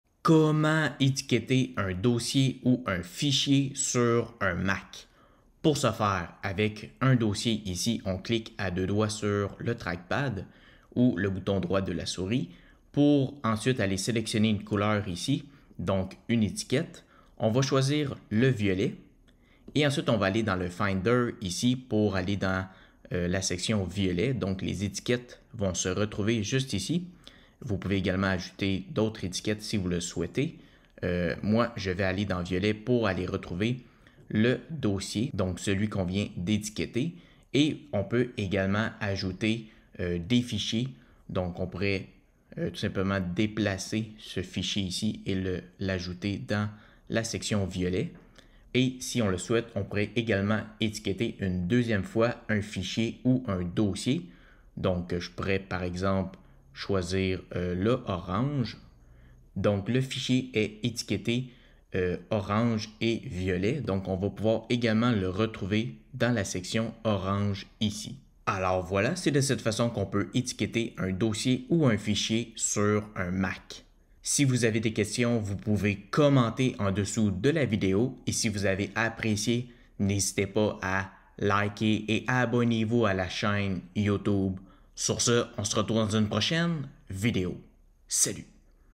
Comment étiqueter un dossier ou un fichier sur Mac
Comment étiqueter un fichier sur Mac  ? Comment mettre une étiquette à un dossier sur Mac ? Découvrez la façon d'ajouter des étiquettes à des dossiers ou des fichiers sur votre Mac.